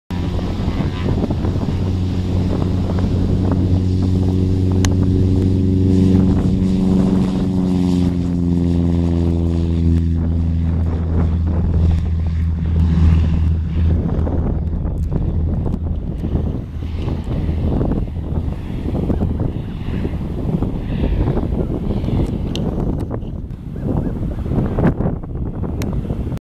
N252YL. 1986 Mooney M20K 252 Aircraft For Sale at Trade-A-Plane.com
N252YL. 1986 Mooney M20K 252, 3800TT, 275 SMOH/SPOH.